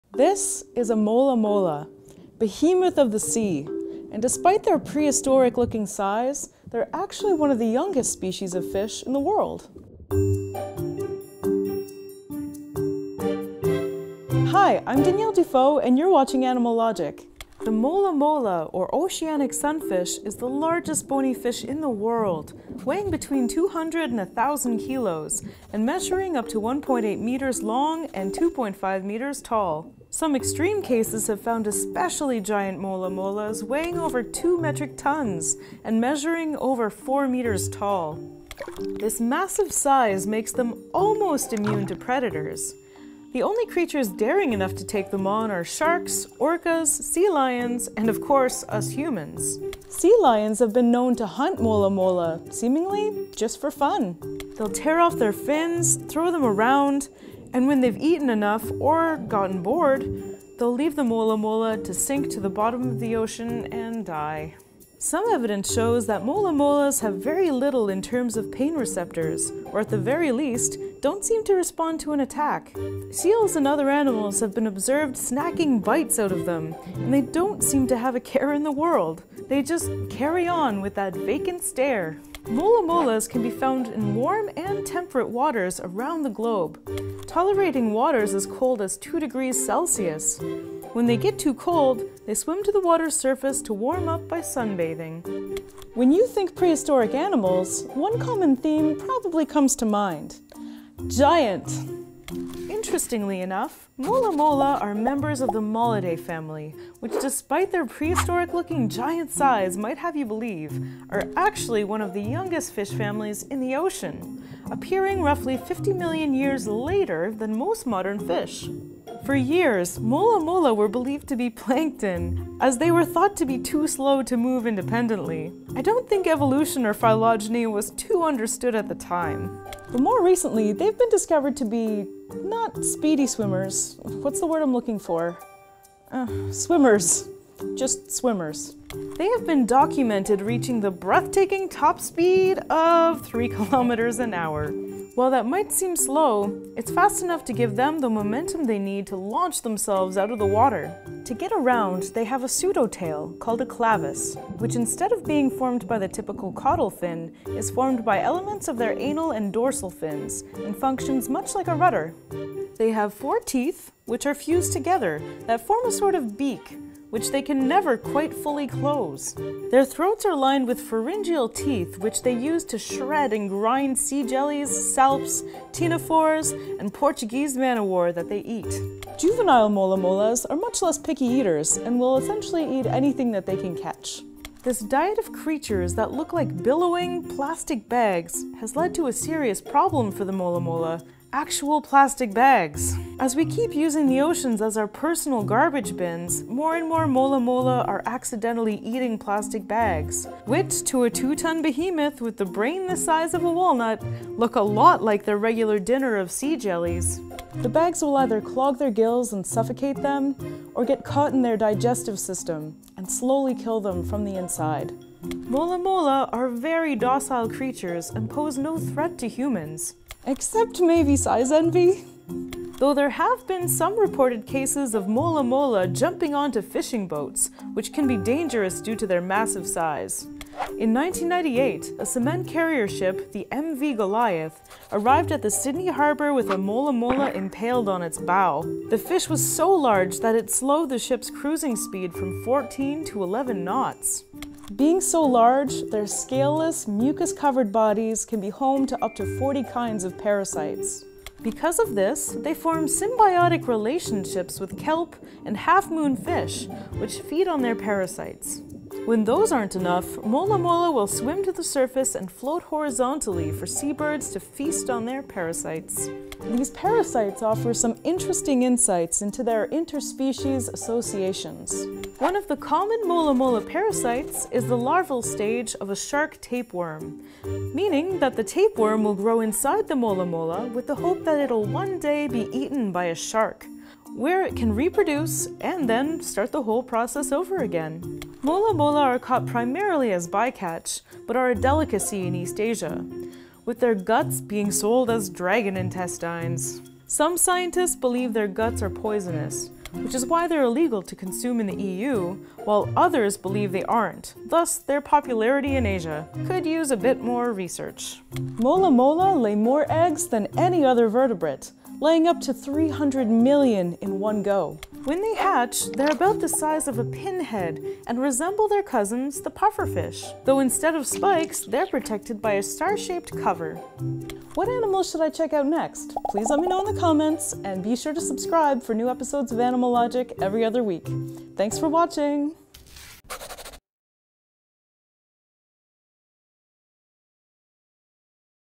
Mola Mola: Behemoths of the Deep
This is a Mola Mola, behemoths of the sea, that despite their prehistoric looking size, are actually one of the youngest species of fish in the world.

CREDITS
Created by Dylan Dubeau and Andrew Strapp
Written, Directed, Shot and Produced by Dylan Dubeau
Hosted by Danielle Dufault
Edited by Jim Pitts
Research by Andres Salazar